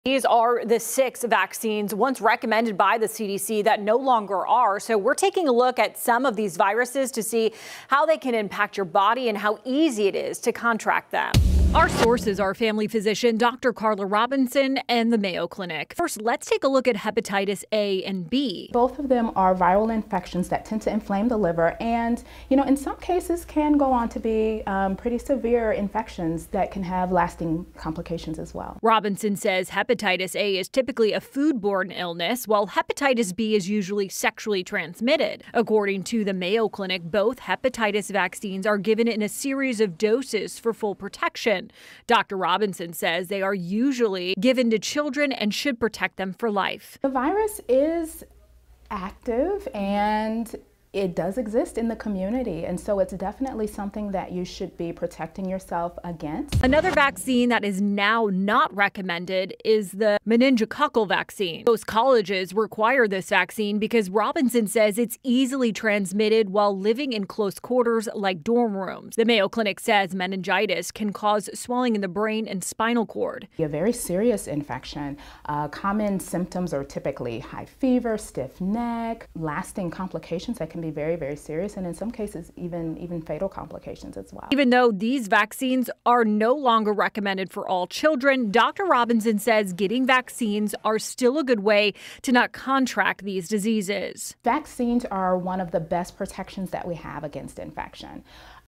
VERIFY
The CDC removed several vaccines off the recommendation list for children. What to know about the infections and diseases on the list.